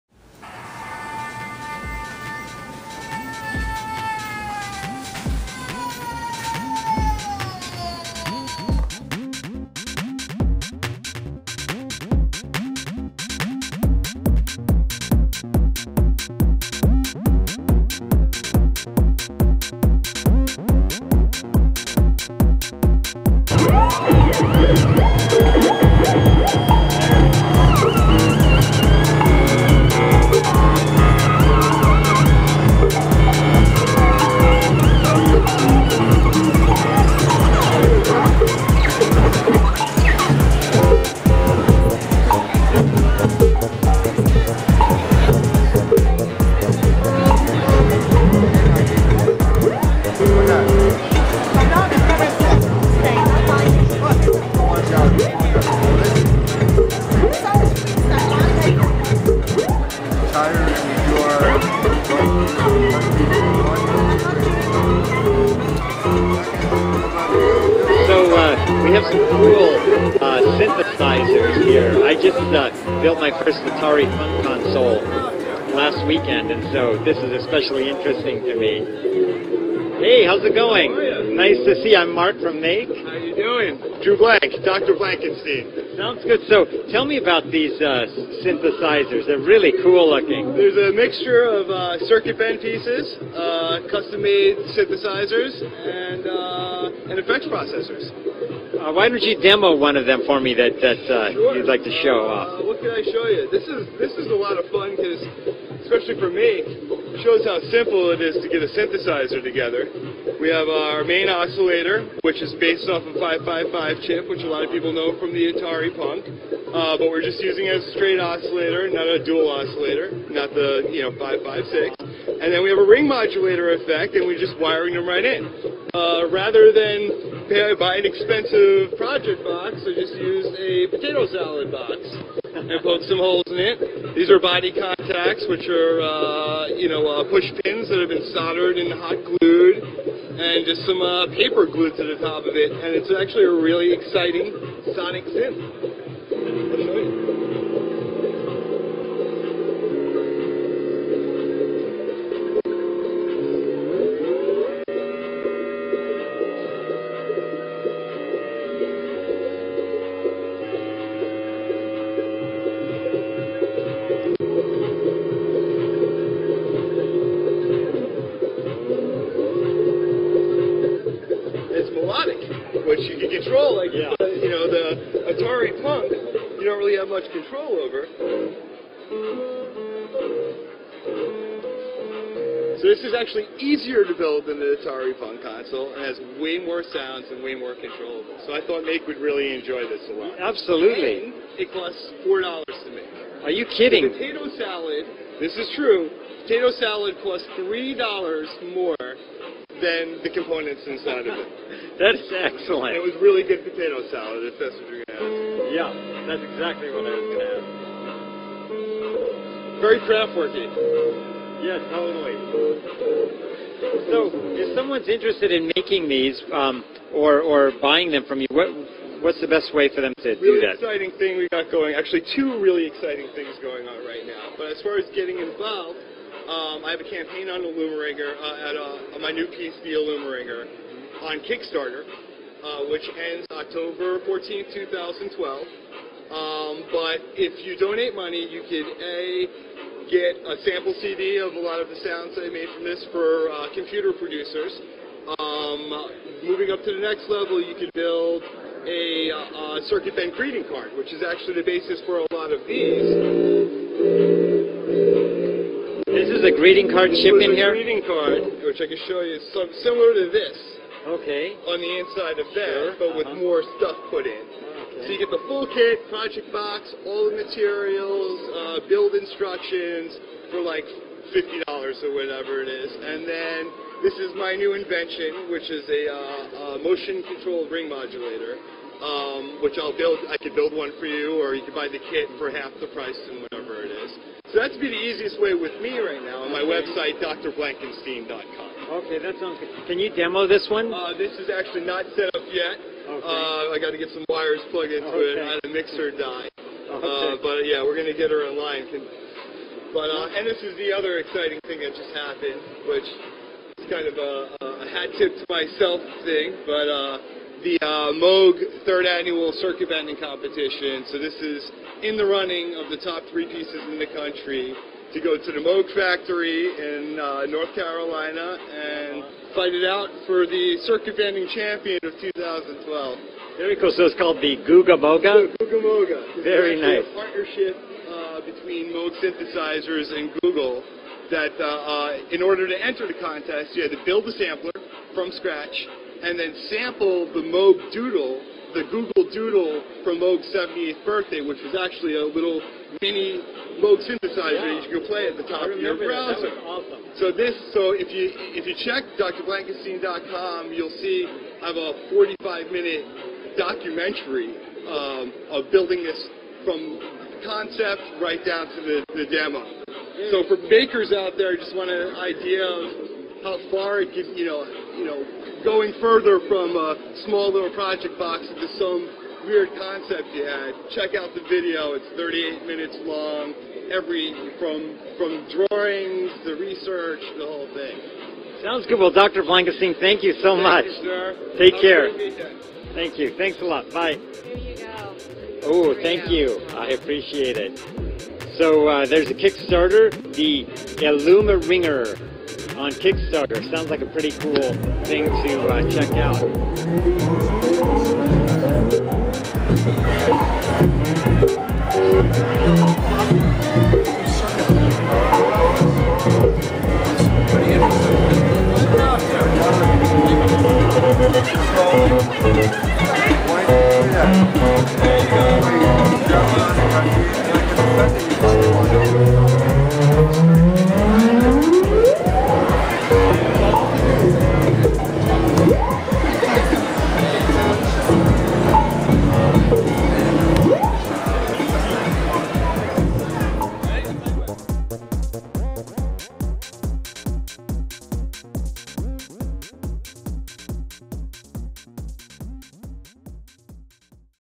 Mark Frauenfelder of BoingBoing interviews Dr. Blankenstein @ World Maker Faire 2012
Mark Frauenfelder editor-in-chief of Make Magazine / creator of BoingBoing.com interviews Dr. Blankenstein at this years World Maker Faire 2012 NYC.

This is a little video I put together from the Maker Faire at the Queens Museum of Science.  It is showing just a few mins of a few of the THOUSANDS of great men, women and children who stopped by to check out some of my custom synthesizers, effects and circuit bent instruments.  The kids LOVED IT!  And we LOVED watching the kids enjoy and learn about the exciting world of circuitry and electronic music.
The video also has a little interview by the one and only Mark Frauenfelder.. editor in chief of Make Magazine and creator of BoingBoing.com (fellow mad scientist) and Dr. Blankenstein.  If I met you there... I want to say hi, and thanks for all your support and for stopping by my table. If you weren't able to go, you can see all the wonderful live footage from the Maker Faire
Can't wait for next year!  Thank you everyone, and THANK YOU MAKE MAGAZINE!  :)